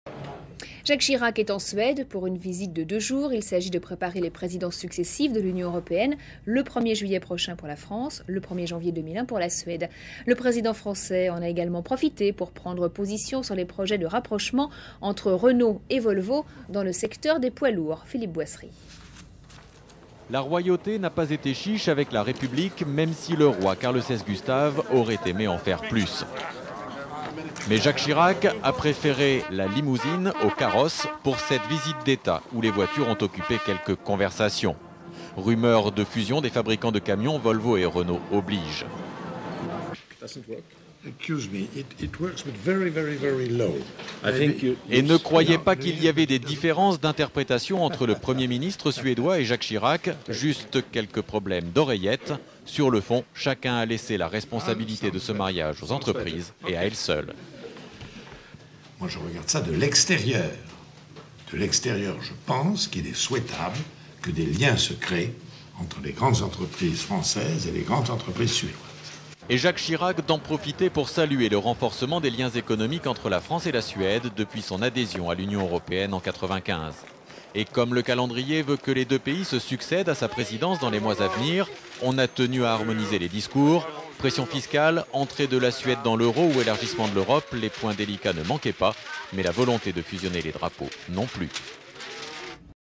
Visite de Jacques Chirac en Suède pour soutenir Renault - Archive INA
10 avril 2000
Jacques Chirac est en Suède pour une visite de 2 jours : il s'agit de préparer les présidences successives européennes, le 1er juillet prochain pour la France, le 1er janvier 2001 pour la Suède ; le Président a pris position sur les projets de rapprochements entre Renault et Volvo dans le secteur des poids lourds.